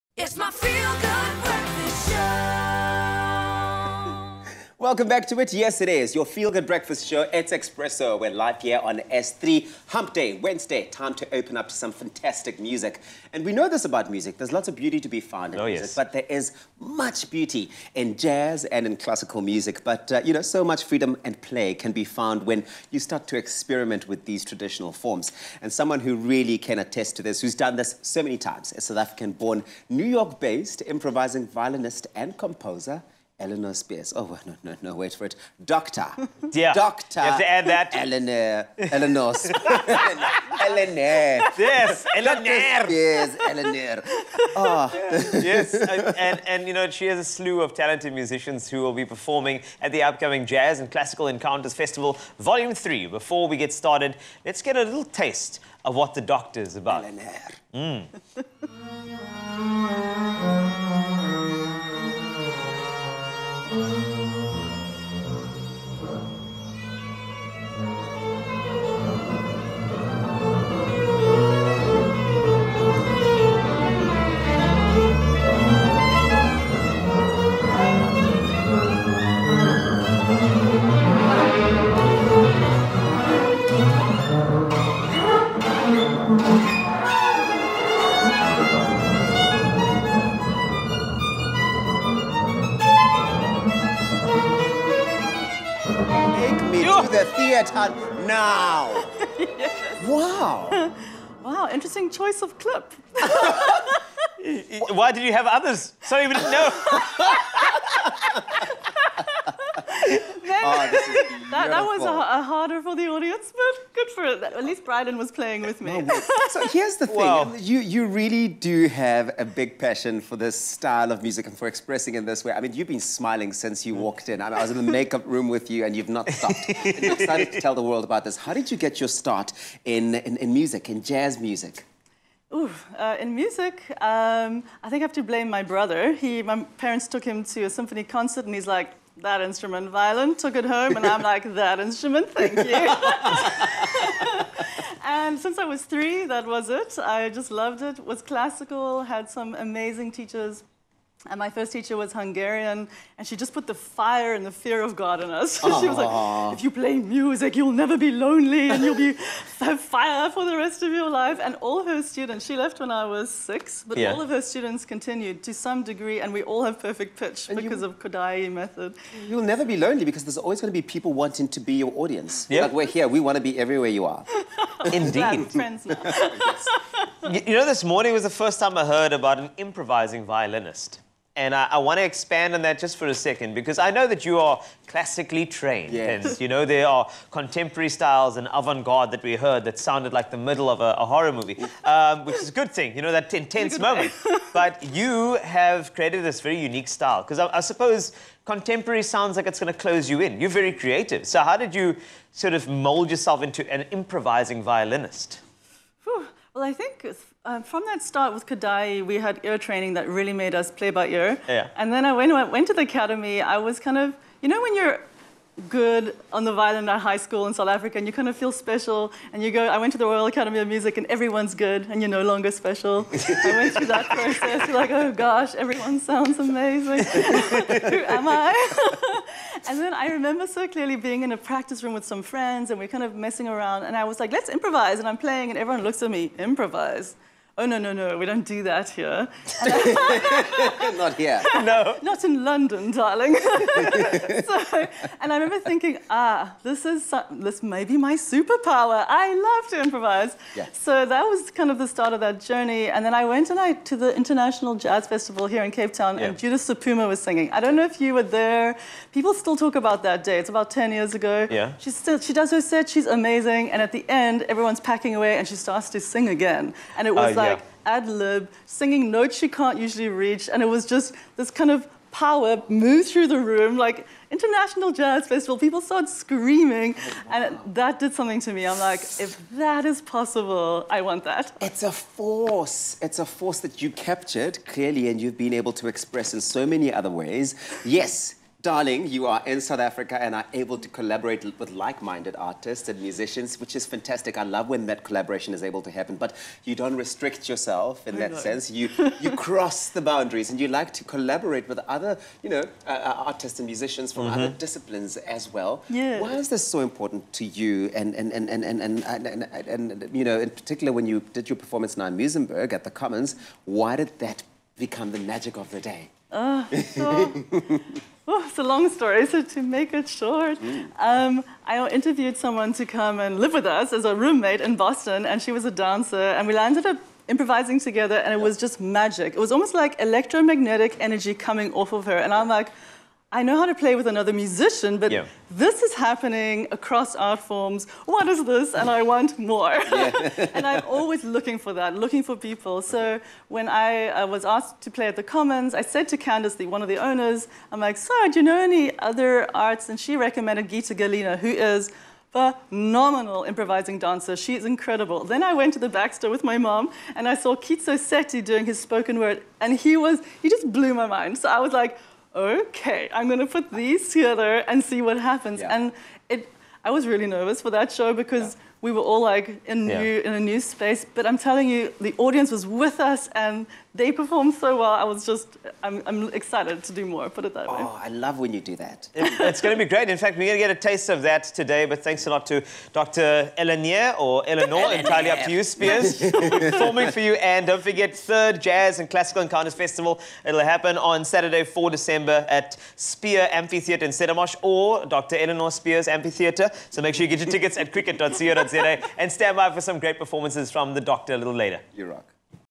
Improvising violinist Elinor Speirs on her career and music
Improvising violinist and composer, Elinor Speirs, joins Expresso to share more about how she got started playing classical violin, and found her improvisational and compositional expression in contemporary and avant-garde jazz. She is performing at the upcoming Jazz & Classical Encounters Festival Volume 3 on Saturday 4 December at the Spier Amphitheatre in Stellenbosch. Tickets are available from Quicket.co.za.